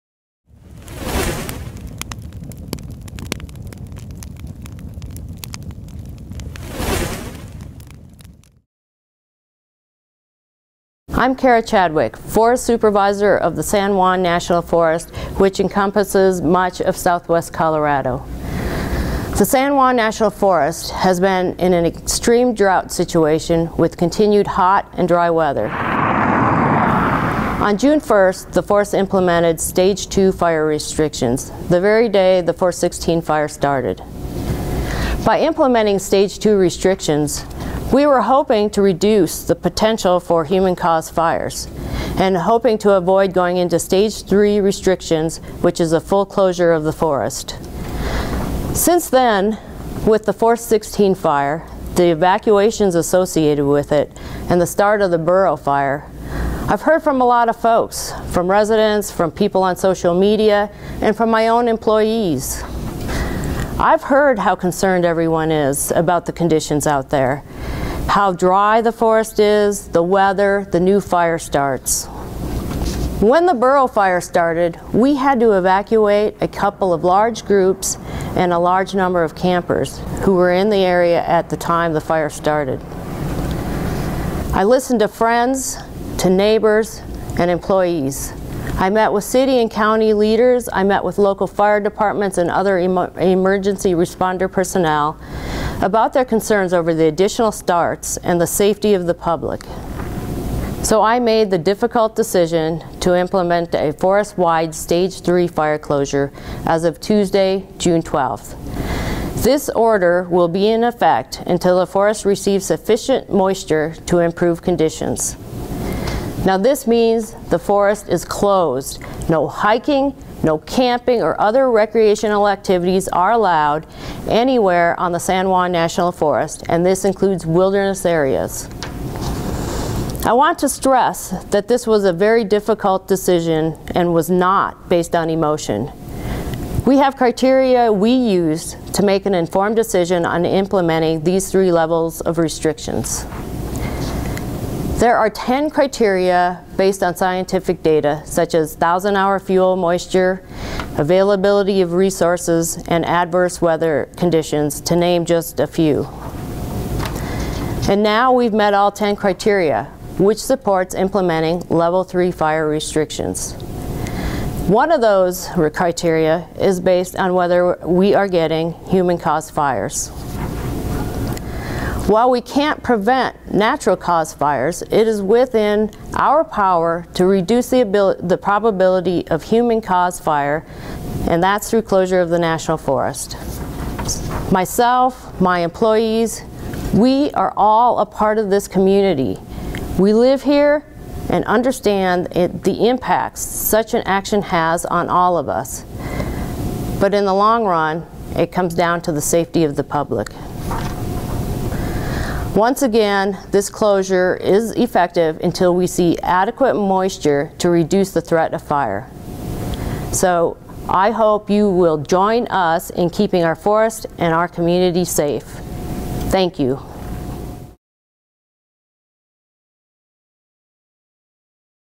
San Juan National Forest: Stage 3 Forest Closure
In June 2018, the San Juan National Forest closed to visitors, due to the extremely dry conditions on the forest and very high to extreme fire danger.
This video was produced for the forest by the Hecker Fire Prevention Team, outlining the decision by forest managers and how they arrived at the Stage 3 declaration- forest closure.
The video was shot by a production crew from the City of Durango Government Television group, and edited by the Hecker Team.
The team worked closely with the City of Durango's TV group during this assignment, sharing resources to ensure widespread dissemination of fire prevention messages.